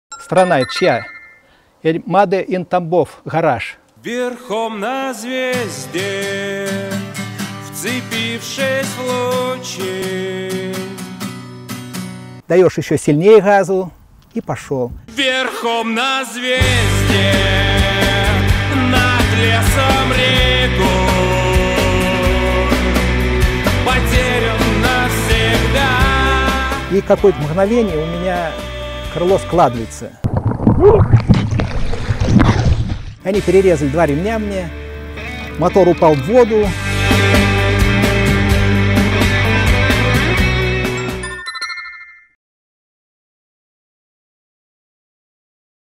Верхом на мечте
По образованию слесарь, а по призванию - увлеченный изобретатель. Тамбовчанин Николай Васильевич Вотановский свою первую технику, машину с ручным приводом, собрал в 4-м классе. Потом были аэросани, снегоход и летательный аппарат!